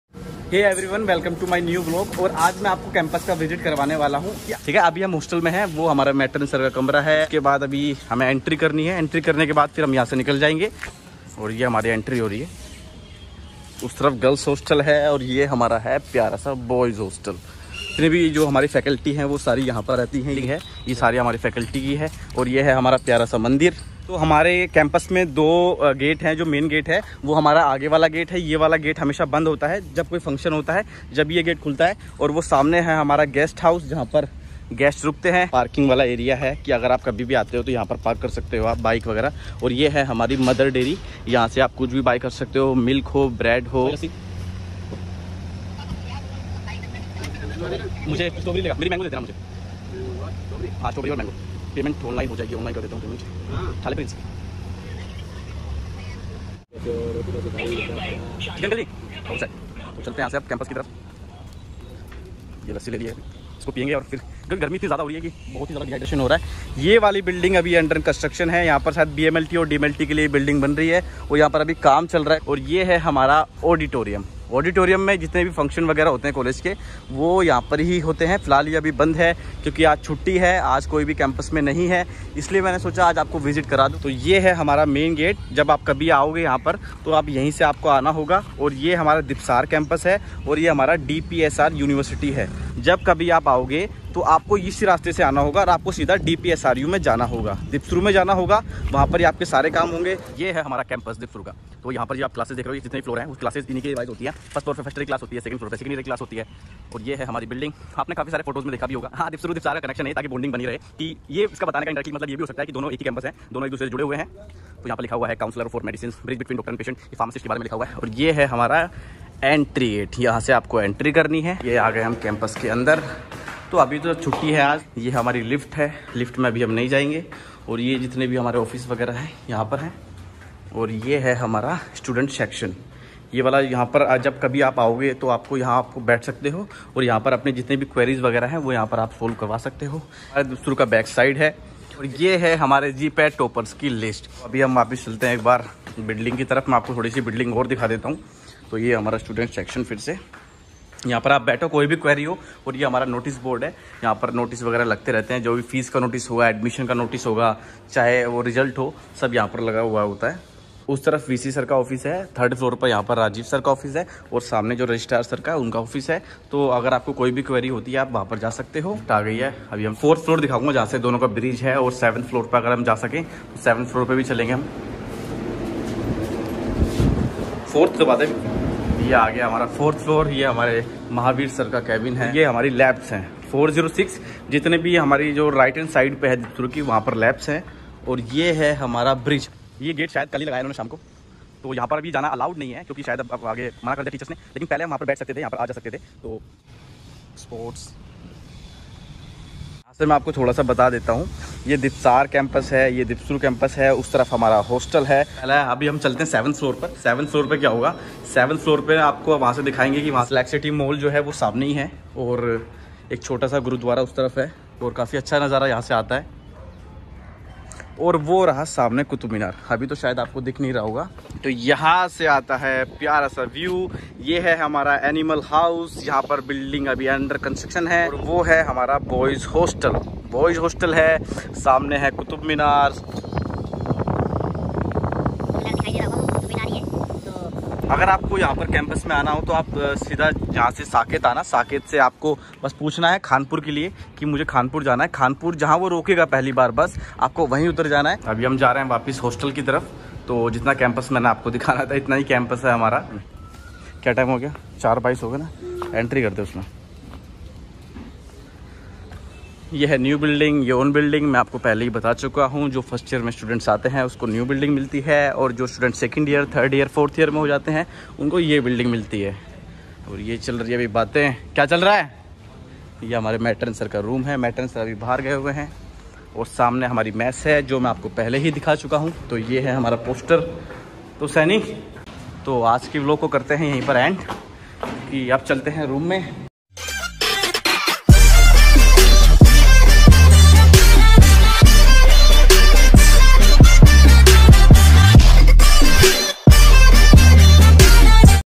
DIPSAR-DPSRU Campus Tour || DPSRU Vlog #02 || Admission 2023-24 || B Pharma Govt College
Hey Everyone
I am A Student Of DPSRU MBA-PM 1st Year